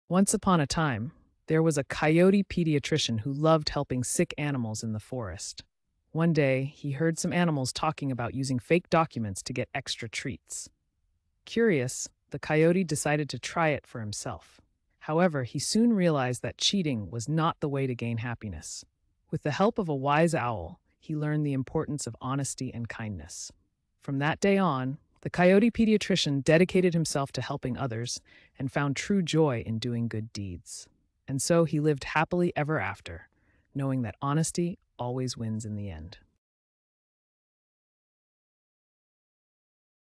The Coyote Pediatrician
Follow the adventures of the Coyote Pediatrician as he learns an important lesson about the consequences of using fake documents or personal data to gain benefits or circumvent the law.